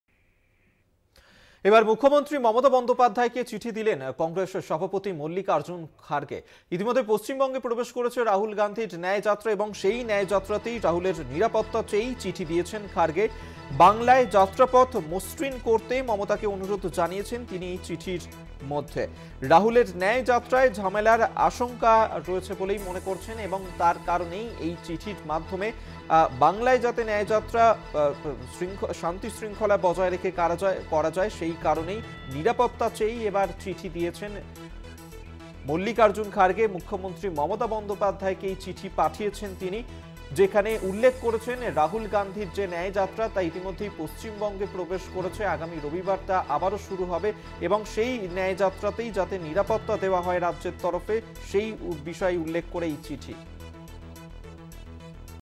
Mamata Banerjee | Mallikarjun Kharge | ন্যায়যাত্রায় নিরাপত্তা চেয়ে মমতাকে চিঠি মল্লিকার্জুন খাড়গের
website link for this copy :
@Kolkata Tv